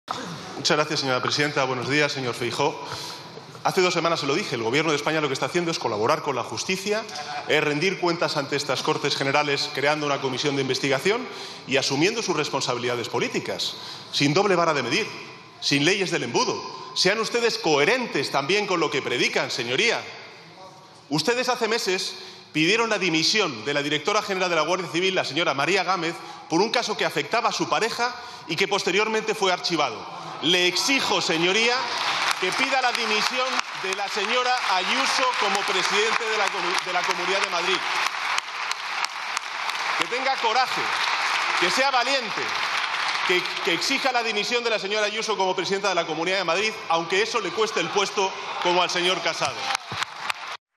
Sánchez exige a Feijóo que pida la dimisión de Ayuso como presidenta de la Comunidad de Madrid
Sánchez exige a Feijóo que pida la dimisión de Ayuso como presidenta de la Comunidad de Madrid: "Tenga coraje aunque le cueste el puesto como al señor Casado"